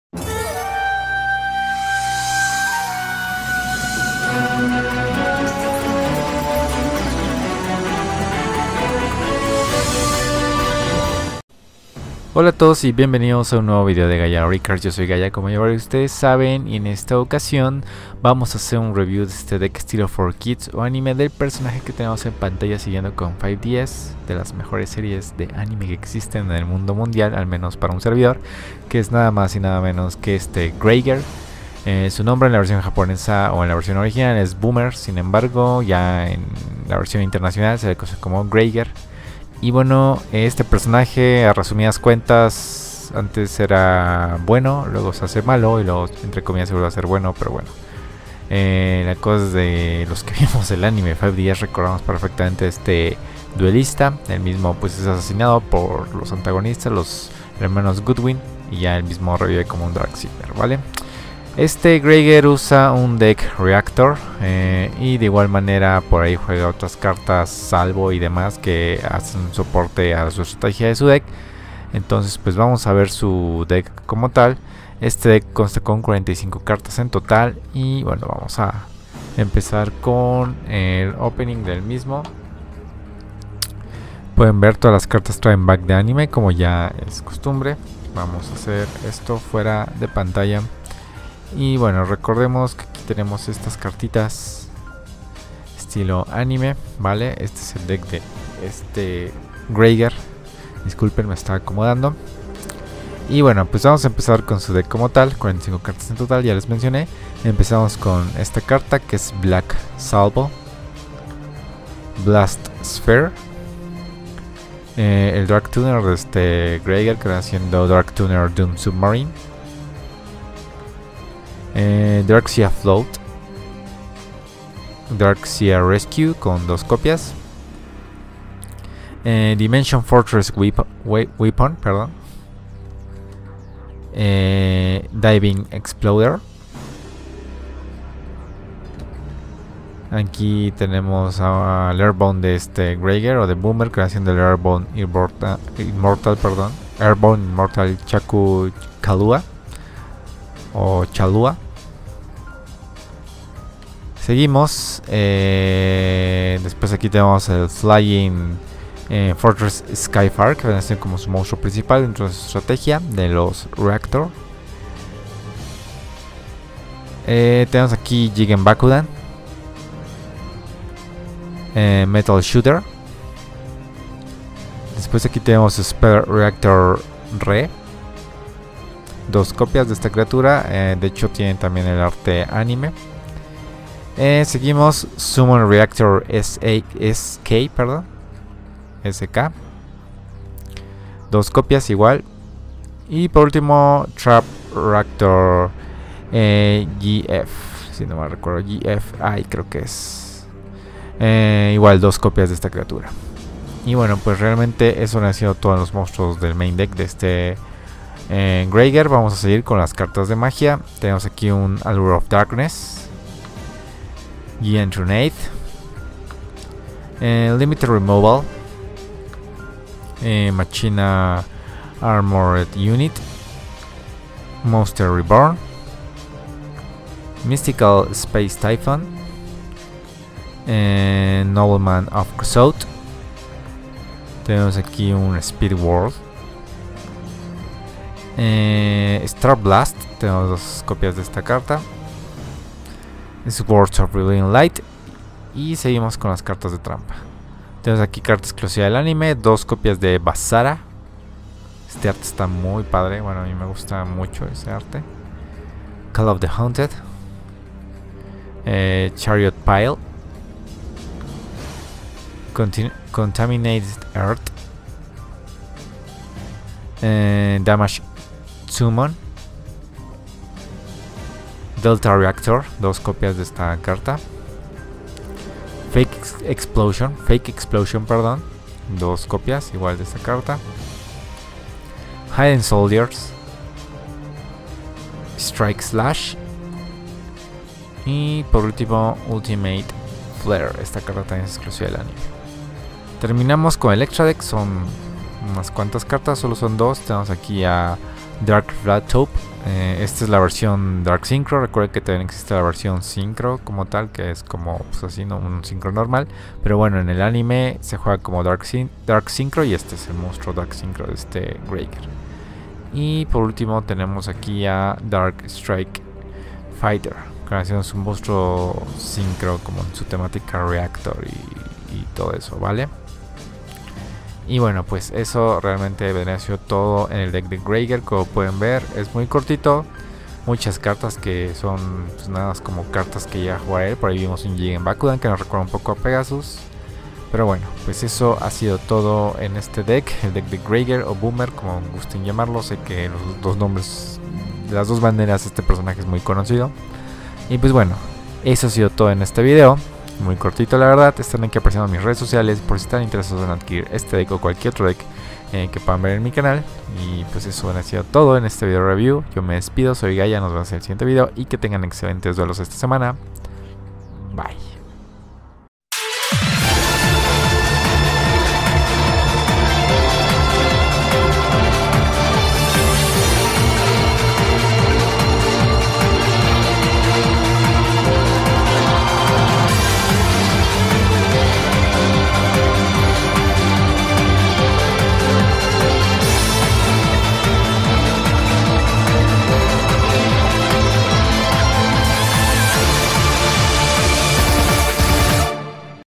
Yu-Gi-Oh! | Greiger Deck | Gaia OriCards
Greiger's Anime Deck Profile (ORICA) with 4Kids Anime Style based on the Anime TV-Show. 45 Cards TOTAL!!! For Sales and more, please send me a pm on my networks below. Thanks!